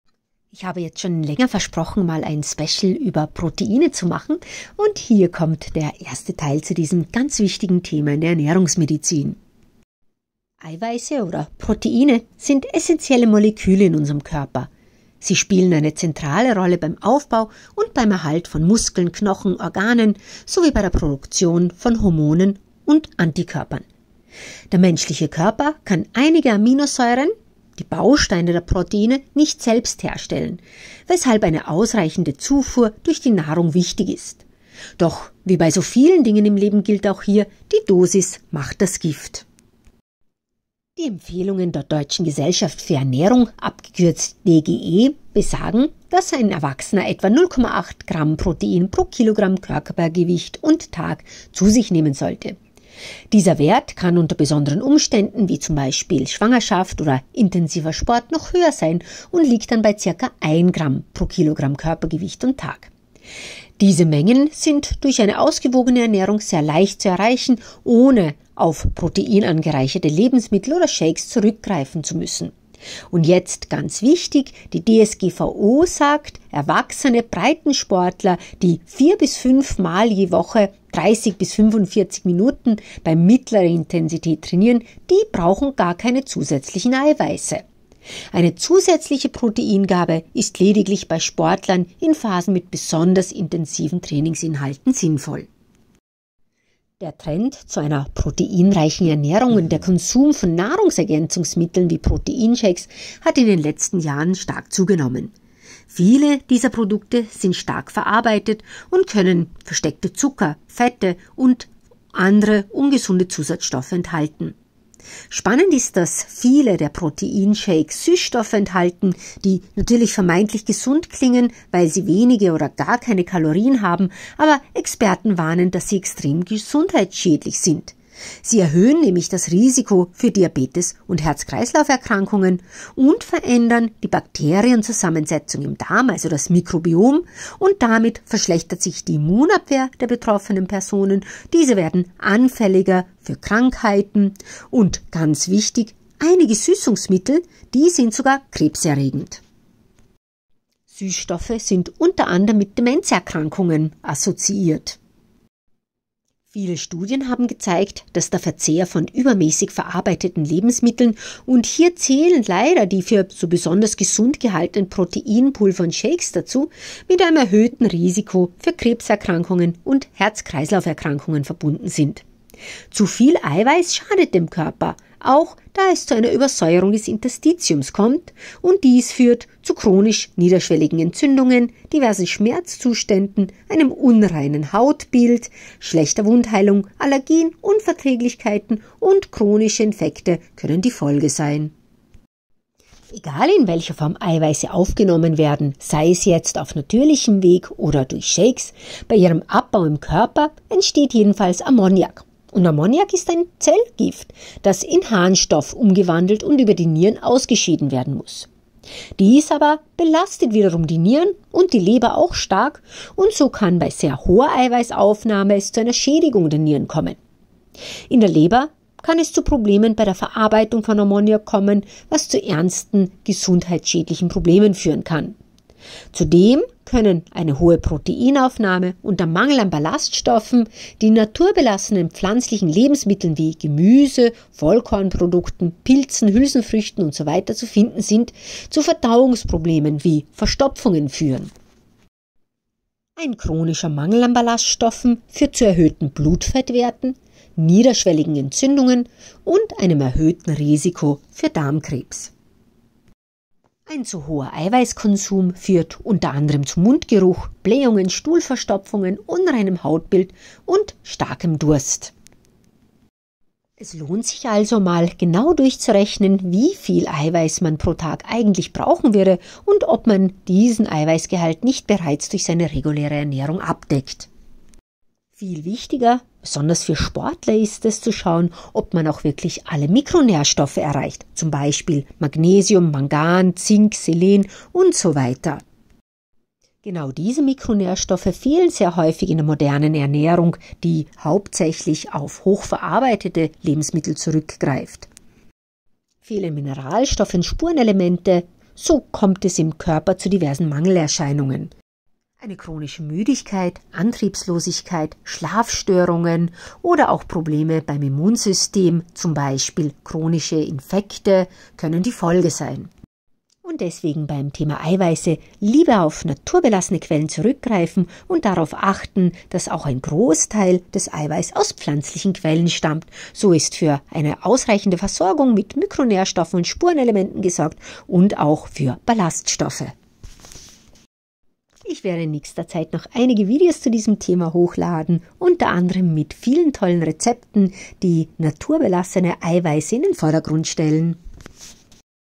Tricks & Tipps für gesunde Ernährung - Ist es notwendig Eiweiß zu substituiern? Trend Proteinreiches
Proteine, auch bekannt als Eiweiße, sind essentielle Moleküle in unserem Körper. Sie spielen eine zentrale Rolle beim Aufbau und Erhalt von Muskeln, Knochen, Organen sowie bei der Produktion von Hormonen und Antikörpern. Der menschliche Körper kann einige Aminosäuren, die Bausteine der Proteine, nicht selbst herstellen, weshalb eine ausreichende Zufuhr durch die Nahrung wichtig ist. Doch wie bei so vielen Dingen im Leben gilt auch hier: Die Dosis macht das Gift.

Die Empfehlungen der Deutschen Gesellschaft für Ernährung (DGE) besagen, dass ein Erwachsener etwa 0,8 Gramm Protein pro Kilogramm Körpergewicht pro Tag zu sich nehmen sollte. Dieser Wert kann unter besonderen Umständen, wie Schwangerschaft oder intensivem Sport, noch höher sein und liegt dann bei ca. 1Gramm/KG . Diese Mengen sind durch eine ausgewogene Ernährung leicht zu erreichen, ohne auf proteinangereicherte Lebensmittel oder Shakes zurückgreifen zu müssen. Laut DGE benötigen Erwachsene Breitensportler, die vier bis fünf Mal je Woche 30-45 Minuten bei mittlerer Intensität trainieren keine zusätzlichen Proteine. Eine zusätzliche Proteingabe ist lediglich bei Sportlern in Phasen mit besonders intensiven Trainingsinhalten sinnvoll.

Der Trend zu einer proteinreichen Ernährung und der Konsum von Nahrungsergänzungsmitteln wie Proteinshakes hat in den letzten Jahren zugenommen. Viele dieser Produkte sind stark verarbeitet und können versteckte Zucker, Fette und andere ungesunde Zusatzstoffe enthalten. Viele enthalten Süßstoffe, die vermeintlich gesund klingen, weil sie wenige oder keine Kalorien haben, aber Experten warnen, dass sie gesundheitsschädlich sind - sie erhöhen das Risiko für Diabetes und Herz-Kreislauferkrankungen und verändern die Bakterienzusammensetzung im Darm, damit verschlechtert sich die Immunabwehr und die Betroffenen werden anfälliger für Krankheiten, einige Süßungsmittel sind sogar krebserregend. Viele Studien haben gezeigt, dass der Verzehr von übermäßig verarbeiteten Lebensmitteln und hier zählen leider die für gesund gehaltenen Proteinpulver und shakes dazu, mit einem erhöhten Risiko für Krebserkrankungen und Herz-Kreislauf-Erkrankungen verbunden ist. Zu viel Eiweiß schadet dem Körper auch da es zu einer Übersäuerung des Interstitiums kommt, dies führt zu chronischen niederschwelligen Entzündungen, diverse Schmerzzustände, einem unreinen Hautbild, schlechter Wundheilung, Allergien, Unverträglichkeiten und chronische Infekte können die Folge sein.

Egal in welcher Form Eiweiße aufgenommen werden, bei ihrem Abbau im Körper entsteht Ammoniak, ein Zellgift, das in Harnstoff umgewandelt und über die Nieren ausgeschieden werden muß. Dies belastet Nieren und Leber stark und so kann bei sehr hoher Eiweißaufnahe zu einer Schädigung der Nieren kommen. In der Leber kann es zu Problemen bei der Verarbeitung von Ammoniak kommen, was zu ernsten gesundheitlichen Problemen führen kann. Zudem können eine hohe Proteinaufnahme und der Mangel an Ballaststoffen, die in naturbelassenen pflanzlichen Lebensmitteln wie Gemüse und Vollkornprodukten zu finden sind, zu Verdauungsproblemen wie Verstopfungen führen. Ein chronischer Mangel an Ballaststoffen führt zu erhöhten Blutfettwerten, niederschwelligen Entzündungen und einem  erhöhtem Risiko für Darmkrebs.
Ein zu hoher Eiweißkonsum führt unter  anderem zu Mundgeruch, Blähungen, Stuhlverstopfung, unreinem Hautbild, starkem Durst